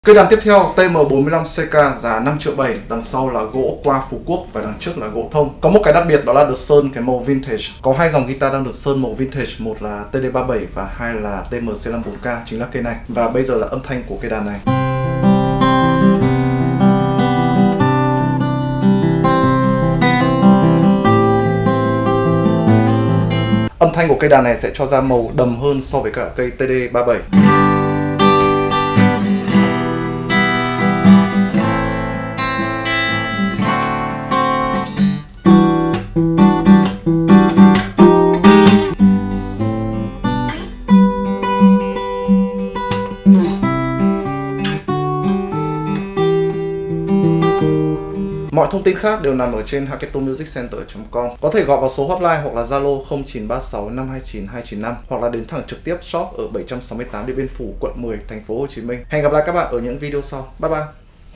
Review nhanh Guitar Trần TM45CK sơn màu vintage, gỗ Koa Phú Quốc, âm thanh rất đẹp!
Review nhanh TM45CK giá 5,7tr, lưng và hông từ gỗ KOA Phú Quốc, âm sắc đều, tròn, đẹp và sáng. Cần đàn dễ chơi, bấm êm ru.